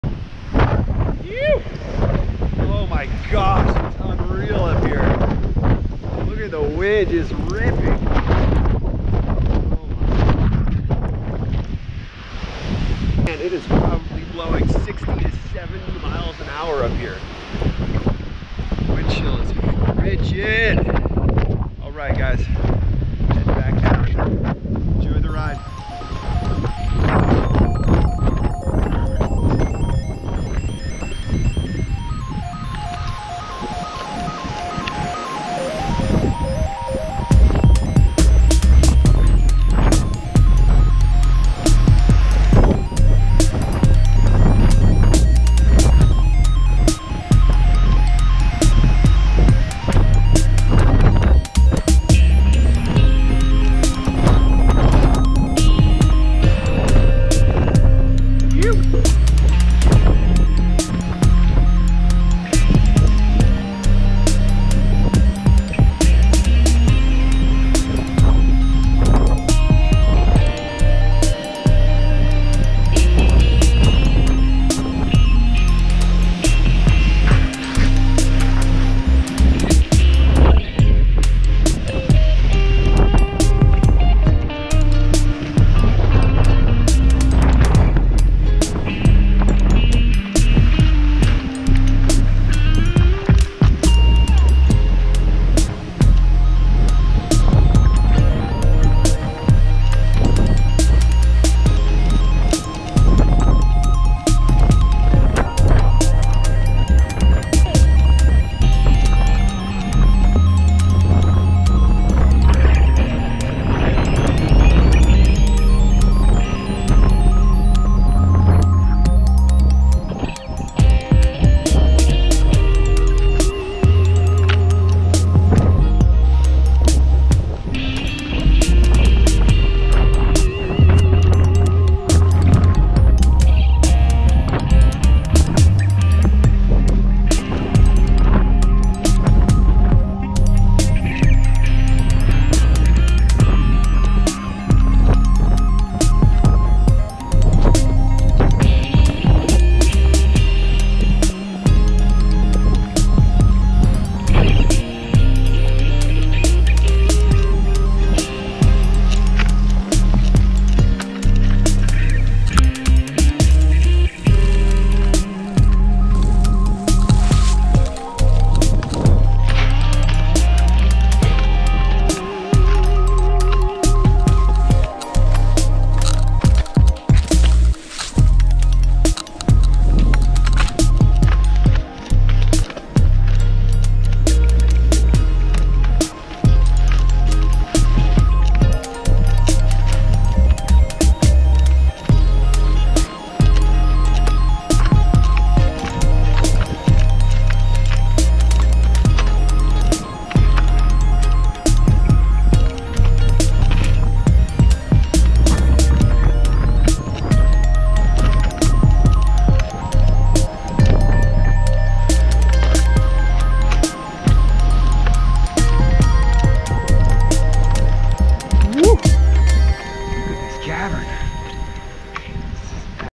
Skiing Down Mt. Mitchell!!!
Sunday, February 2nd. Ascended Mt. Mitchell Trail to Summit. Skied down until snow ran out.